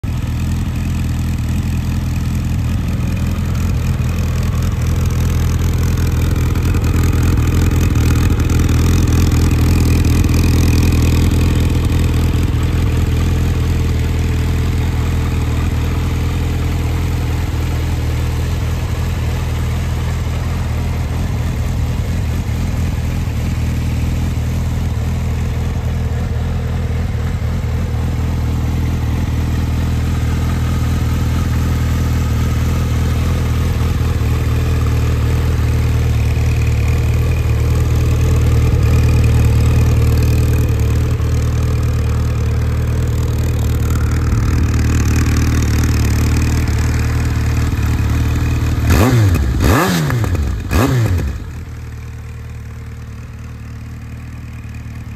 Used Yamaha XJR1300 at Guildford Harley-Davidson with SC Project Exhaust
Guildford Harley-Davidson are pleased to offer this 2016 Yamaha XJR1300 in Grey. It is in fantastic condition. Features the OHLINS suspension. Fitted with an SC Project can to give it a lovely exhaust note without being overbearing.